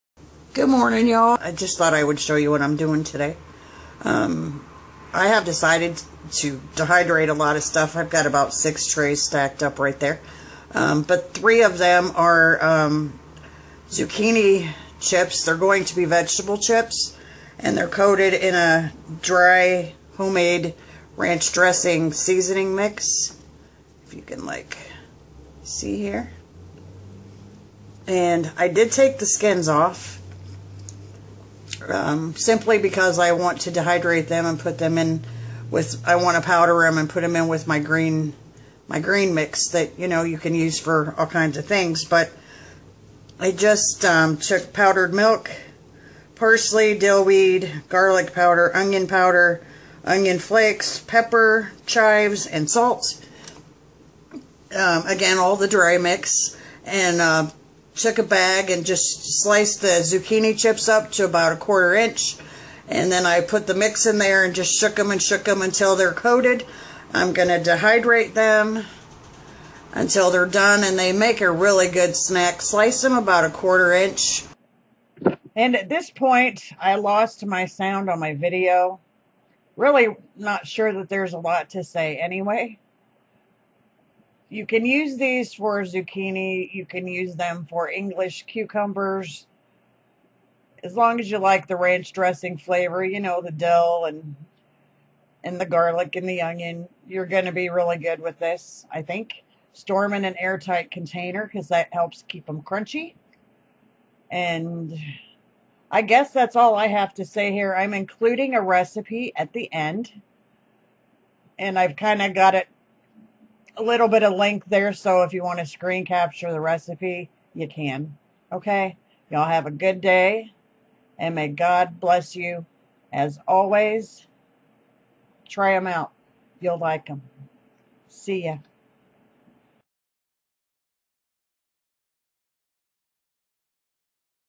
Dry Ranch Mix over Zucchini
Easy Veggie Chips. Interchange for your favorites! I really must stop saying Um!!! God bless y'all.